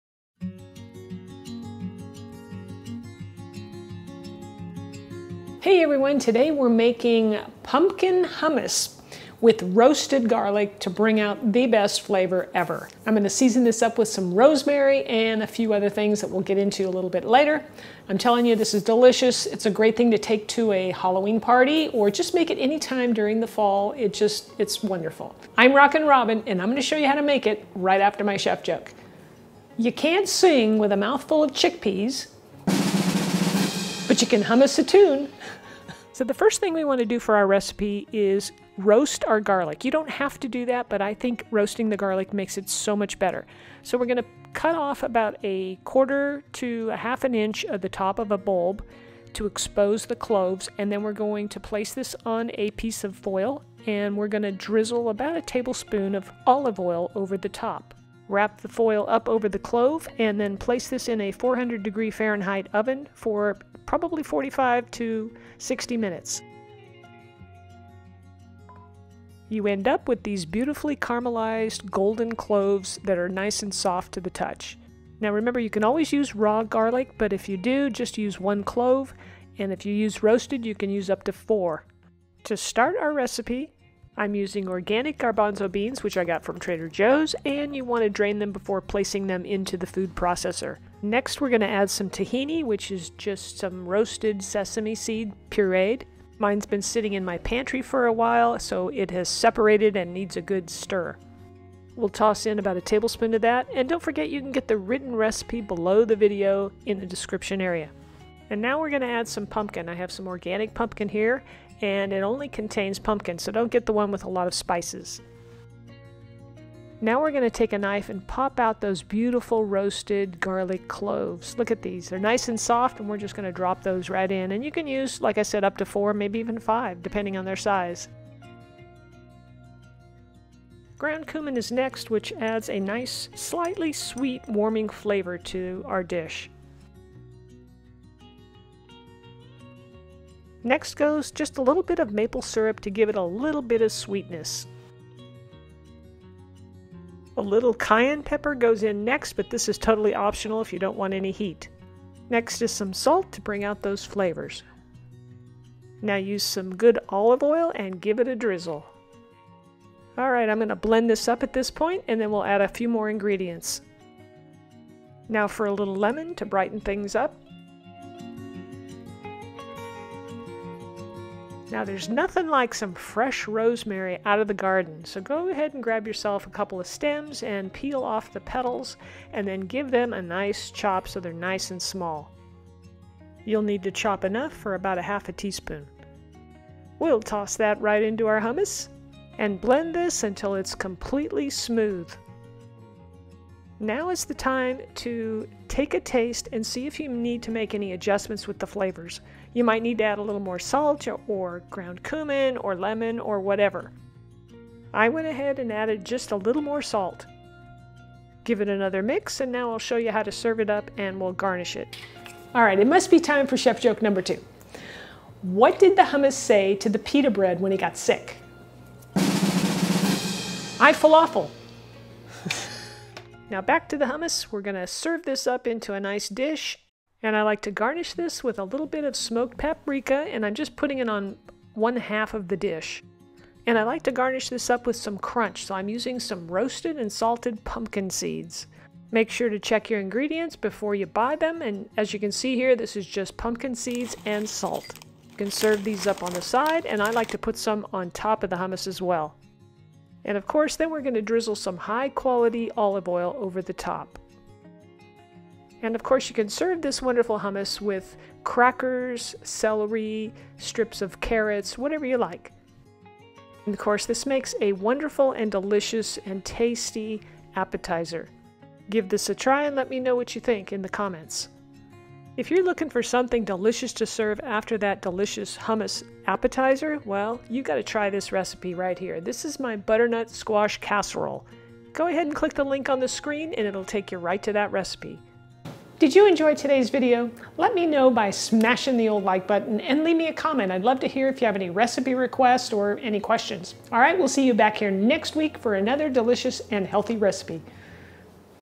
How To Make Delicious Pumpkin Garlic Hummus | Vegan and Gluten Free Recipe.
Not sure how to make pumpkin garlic hummus? This video is for you! We'll show you how to make this delicious and healthy dish in just a few simple steps. Not only is this pumpkin hummus perfect for fall, but it's also vegan and gluten-free, so it's perfect for any dietary needs!

Kitchen Tools I Like:
▶︎ Tortilla Warmer

▶︎ Wooden Spoons

▶︎ Vitamix 5200

▶︎ Cooling Racks

▶︎ Arrowroot Powder

▶︎ Taco Holders

Pumpkin Garlic Hummus
Ingredients:
1 can (14 oz) pumpkin puree
1 can (14oz) garbanzo beans, drained
1 tsp. Ground cumin
2 tsp. Maple syrup
1 Tbsp. tahini
⅛ tsp. Cayenne pepper
1 Tbsp. fresh lemon juice
½ tsp. Fresh rosemary, chopped fine
1 Tbsp. quality olive oil
½ tsp. Salt or more to taste
4 - 5 cloves of roasted garlic, or 1 to 2 raw garlic cloves, minced

Garnish:
Smoked paprika
Roasted pumpkin seeds
1 Tbsp. quality olive oil

Take 1 bulb of fresh garlic and cut off ¼ to ½ inch of the top of the bulb to expose the cloves. Place the cut bulb on a piece of foil and pour a good tablespoon of olive oil over the top of the exposed cloves. Wrap the garlic up in the foil and place in the oven for a good 45 - 60 minutes or until soft and a beautiful golden brown.

Once the garlic is done, remove 4 or 5 cloves with a knife. They should pop right out. Place them in a food processor along with the garbanzo beans, pumpkin, 1 Tbsp of olive oil, tahini, maple syrup, cumin, cayenne pepper, and salt. Blend the hummus.

Add the lemon juice and chopped rosemary and blend until completely smooth. Now is the time to do a taste test and adjust the flavors as needed.

Pour the hummus into a serving dish and top with a sprinkle of smoked paprika, pumpkin seeds and a drizzle of olive oil. This dip can be served with cut up veggies like carrots and celery and different types of crackers. Enjoy!

Rockin Robin
P.S. Please help me spread the word about my channel.